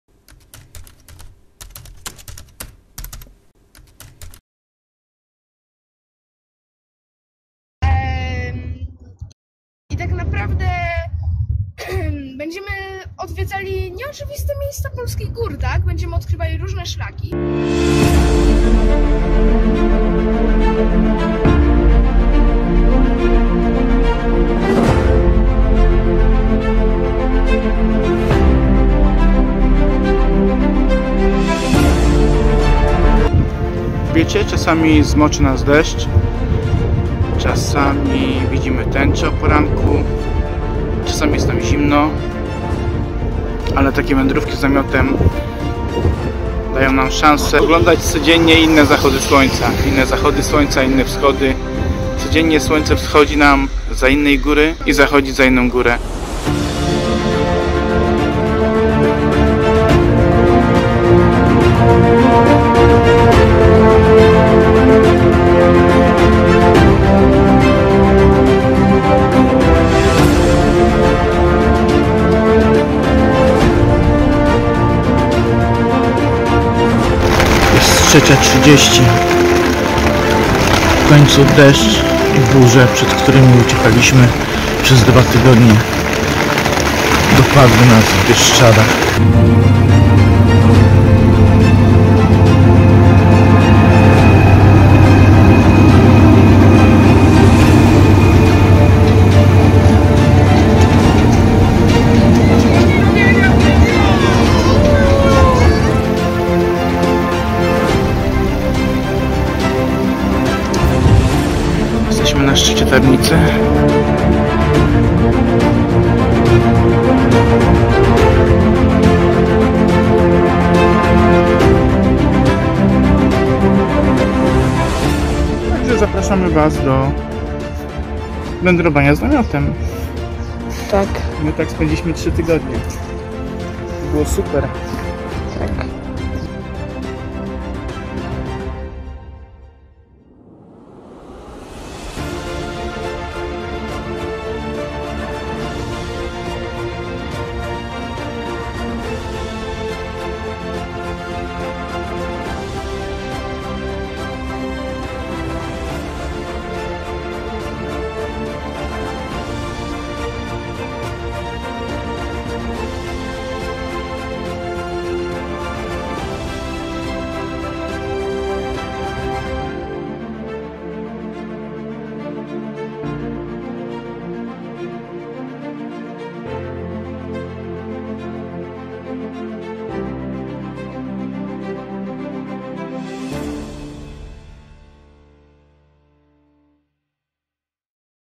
Zakończenie tripu natinaszlaku | dziękujemy że z nami byliście❤
Dzisiaj nadszedł ten dzień kiedy kończymy projekt natinaszlaku.

Trasa od Kotliny Kłodzkiej po Bieszczady, odwiedziliśmy kopalnie, opuszczone miejsca, atrakcje przyrodnicze i miejsca o których być może nawet nie słyszeliście. Wszystko to pokazywaliśmy Wam codziennie na Instagramie, facebooku i po zmontowaniu na youtube. Dziękujemy że wyruszyliście z nami w wakacyjny trip przez góry. Pozdrawiamy z dna serducha.

Ogromne podziękowania dla @PodKrawatem za pomoc przy oprawie graficznej.

Projekt planujemy kontynuować pod tą samą frazą w nasze wszystkie wypady.

Utwory należą do ich prawnych właścicieli i zostały wykorzystane wg prawa cytatu [art.29 ust.1 ustawy o prawie autorskim i prawach pokrewnych